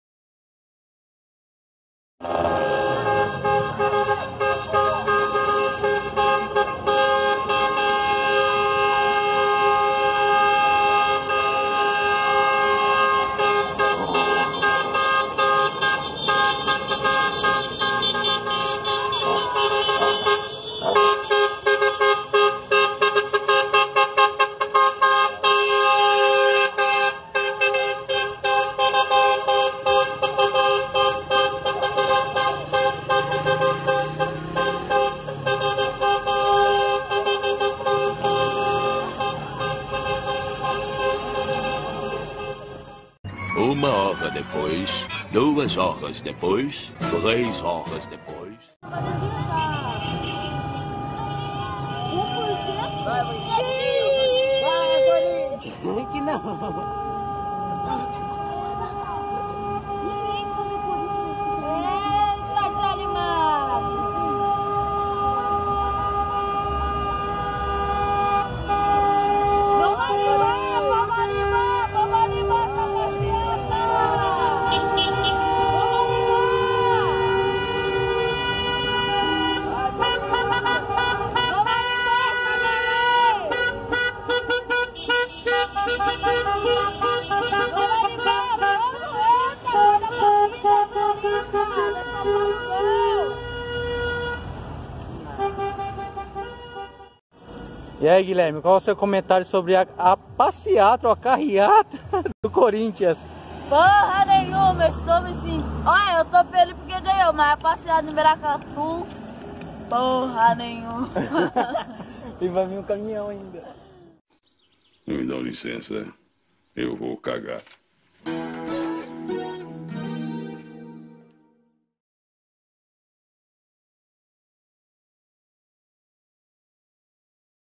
Corinthians campeão paulista de 2019
Carriata igual a essa eu nunca vi com tanta animação e alegria
Vai Corinthians